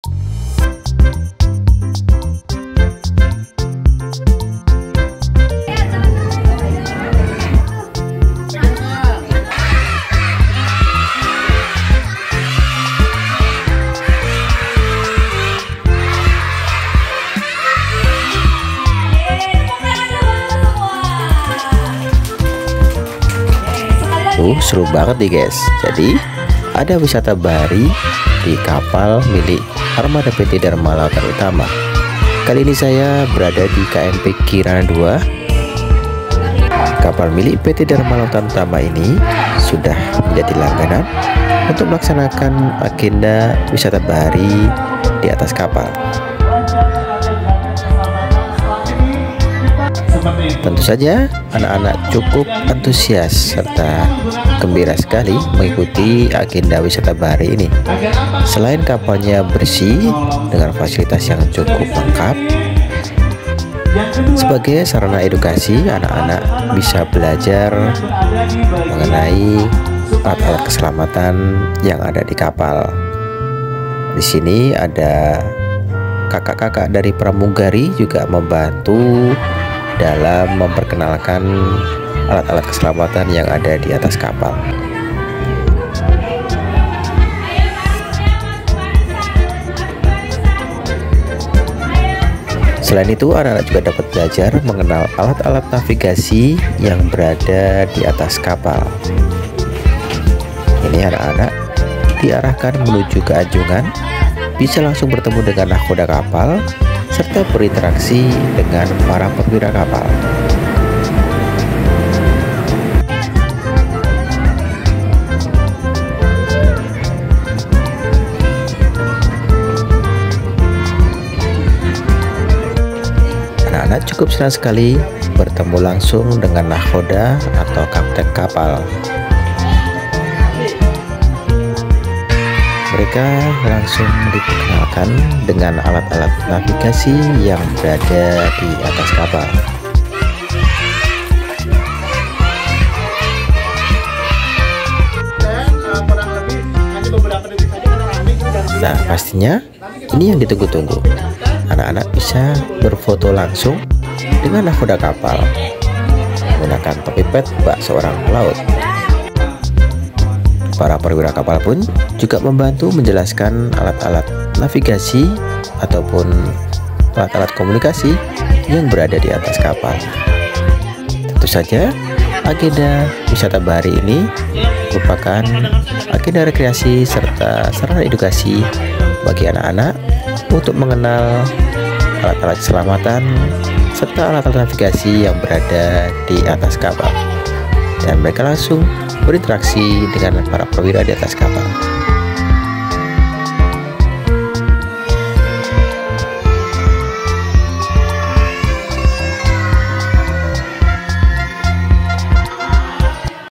Wisata Bahari di atas Kapal Ferry
Agenda wisata bahari menjadi langganan kapal milik PT. Dharma Lautan Utama cabang merak, Sebagai sarana edukasi anak anak diajak berkeliling kapal dan diperkenalkan berbagai macam alat keselamatan dan navigasi, seru banget nih naik kapal KMP Kirana II, kapal bersih dan fasilitas lengkap

wisata bahari di kapal
naik kapal ferry
belajar di atas kapal
kapal merak bakauheni
Pelabuhan merak